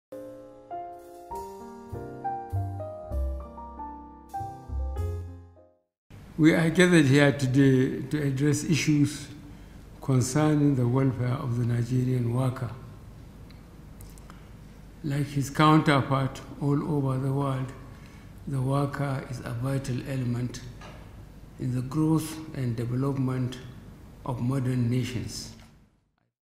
NANTV: Are workers important in Nigeria?
President Buhari says workers contribute to devt.